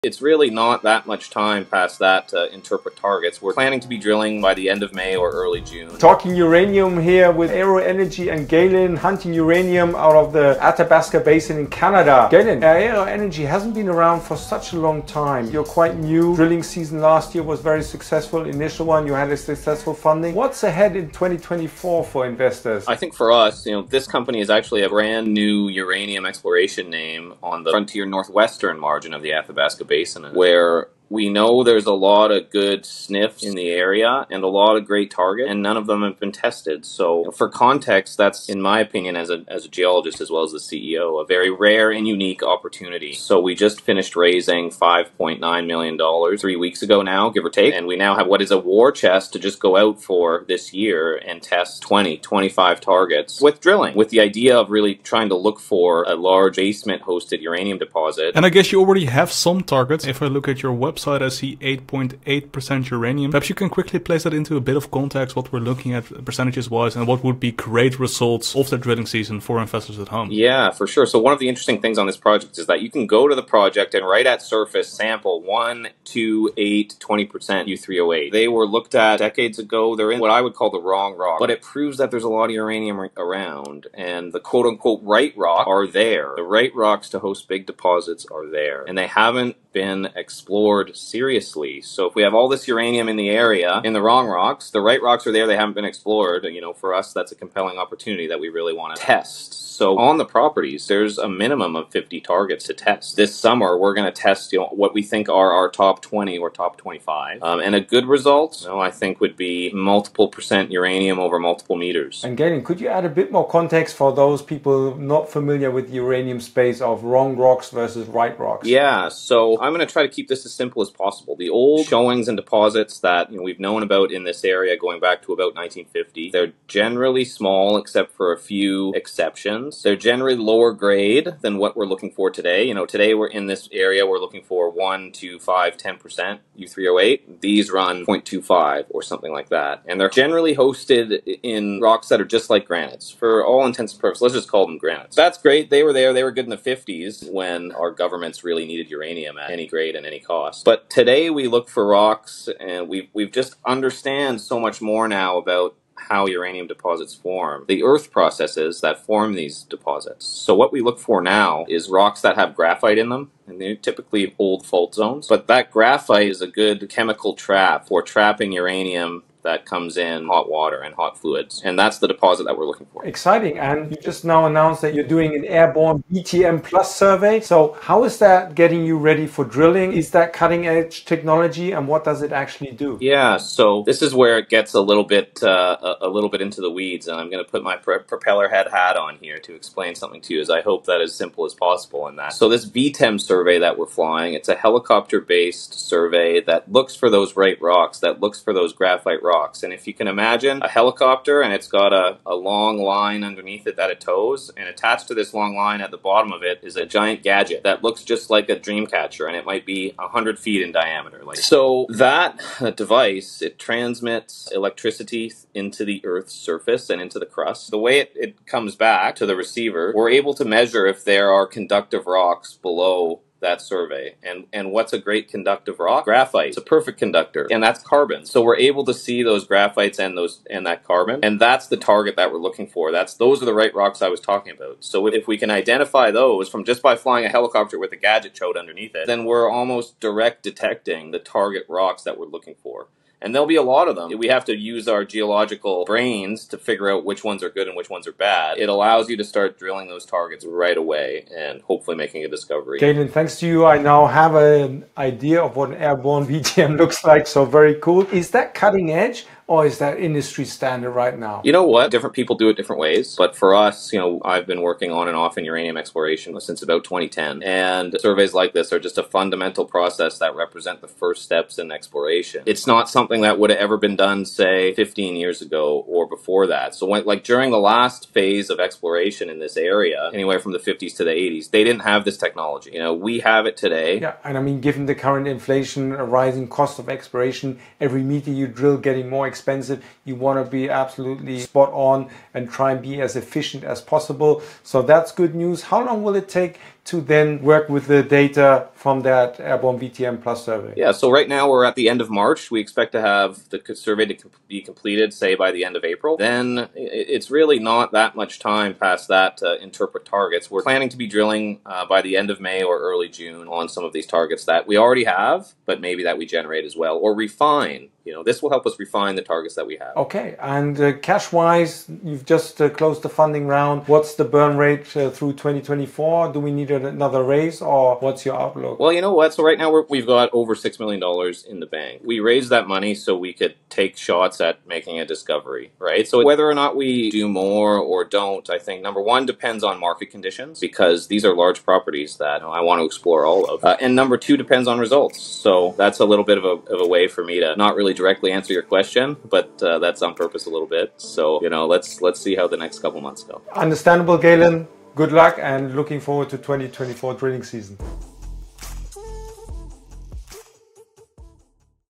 Catalyst: What You Can Expect from Aero Energy in 2024 | $AERO Stock
Talking with the CEO of Aero Energy About Their Uranium Asset in the Athabasca Basin (Canada), Recent Funding and Planned Drilling Campaign, Enjoy!

00:00 Intro
00:20 Company Overview
00:58 2024 Drilling Campaign
01:52 What Rocks are We Looking For?
02:53 Planned Airborne Survey
04:51 When can we Expect Results?

Aero Energy Ltd. | Exploring Uranium Projects in Saskatchewan, Canada
Aero Energy Ltd. is an exploration company focused on the exploration of uranium in Saskatchewan, Canada.

Stock Listings:
📈 TSXV: AERO